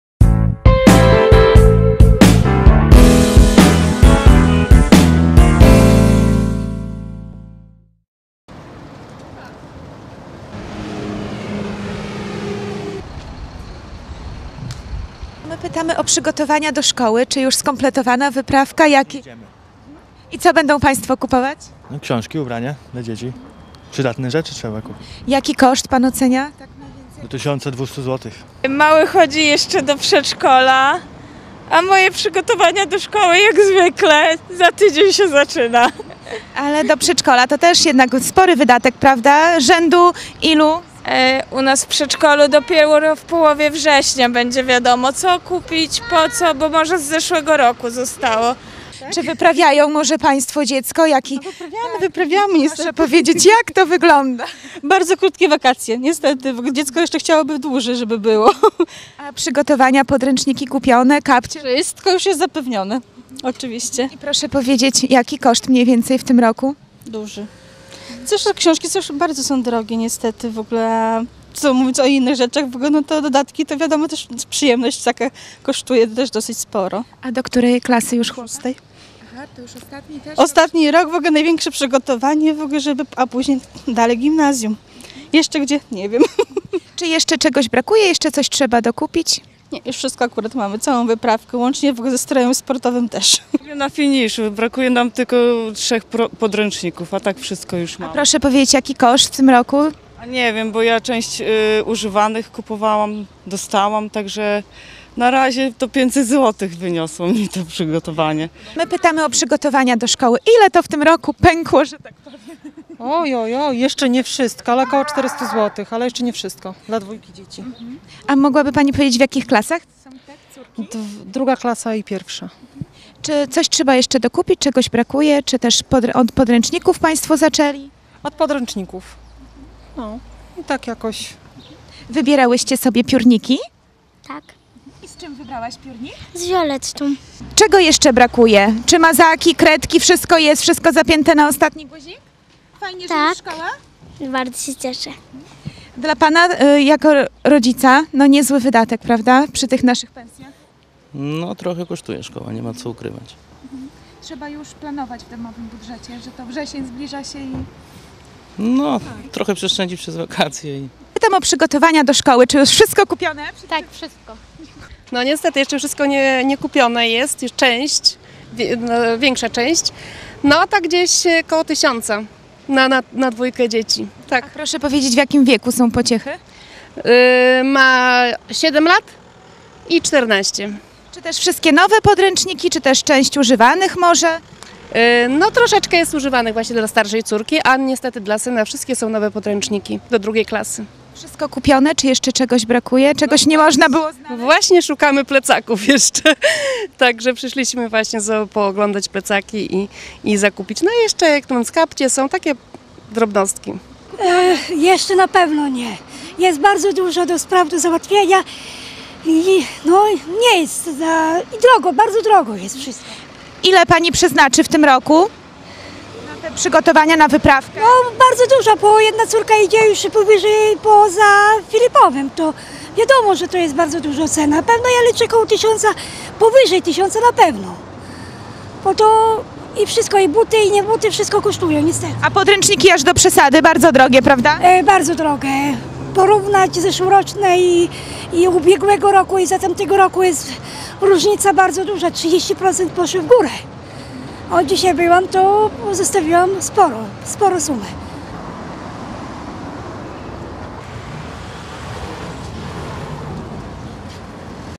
Dzieci idą do szkoły, rodzice płaczą i płacą
Mieszkańcy Suwałk i regionu opowiadają o kosztach wyprawek szkolnych.